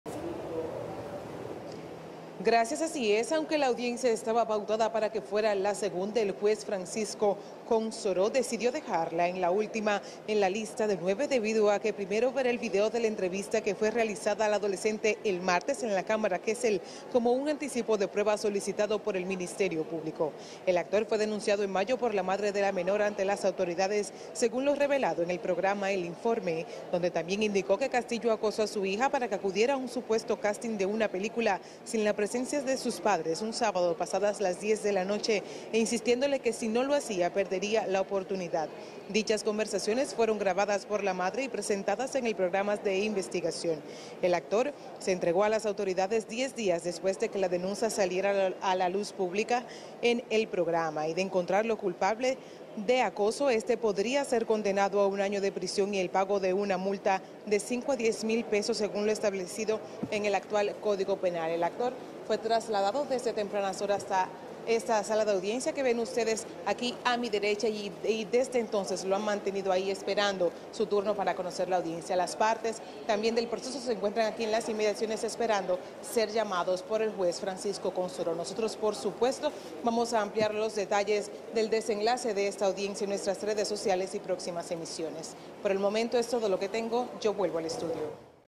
Tribunal se apresta a conocer coerción contra actor Andrés Castillo acusado de acoso
La Oficina de Atención Permanente del Distrito Nacional intenta  conocer por segunda ocasión la medida de coerción contra el actor Andrés Castillo por presuntamente haber acosado a una menor de 14 años.